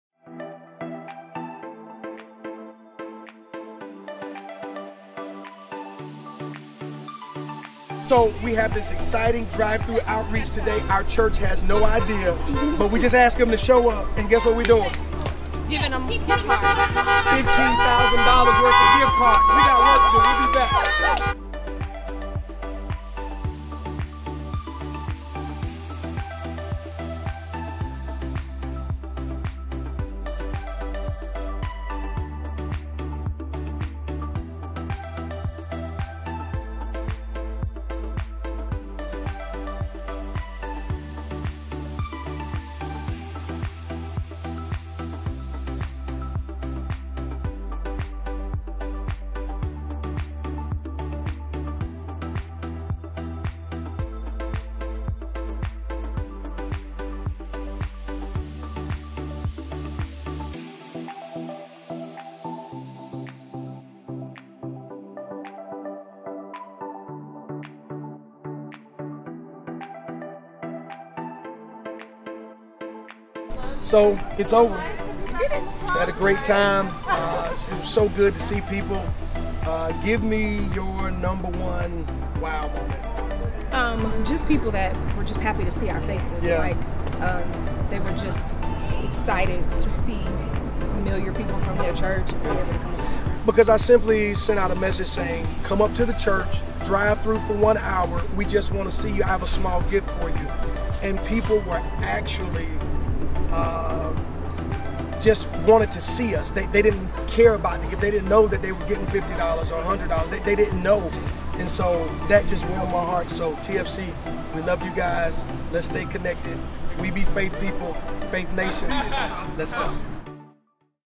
$ 15,000 GIVEAWAY @ The Faith Center
This past weekend, Pastor Vincent Campbell had a CRAZY IDEA to bless the Partners of The Faith Center with $15,000 worth of gift cards 💳, through a Drive By event to say hello 👋🏾
TFC partners had NO IDEA about the 🎁 that was about to be blessed to them. Their reactions? 𝙋𝙍𝙄𝘾𝙀𝙇𝙀𝙎𝙎!!!
We greatly appreciate and thank all the partners who have served and to all that has been a part of our Faith Center Family! Let's continue to give, inspire, and BeMore